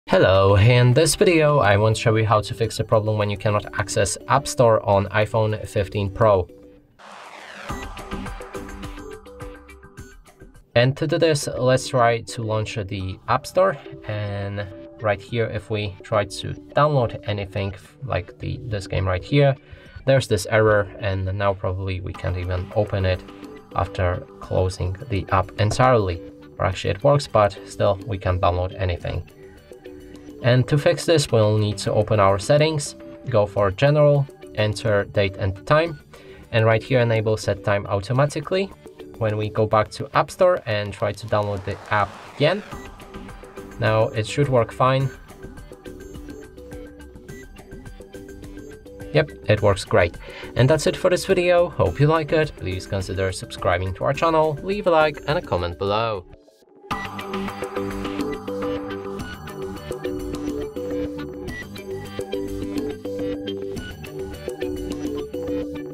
Fix App Store Not Working On iPhone 15 Pro
You Can Find More Useful Tutorials About iPhone 15 Pro On Our
Learn how to troubleshoot and fix issues with the App Store not working on your iPhone 15 Pro. This comprehensive guide provides step-by-step solutions to resolve common problems that may prevent you from accessing and downloading apps.

What could be causing the App Store not to work on my iPhone 15 Pro?
How can I diagnose and fix App Store issues on my device?
Are there common troubleshooting steps I should follow to resolve this problem?
Can you provide specific instructions for checking network connectivity and data usage?
Are there settings or preferences that might affect the App Store's functionality?
What should I do if I encounter error messages or cannot download apps from the store?
Are there updates or software-related issues that could be affecting the App Store?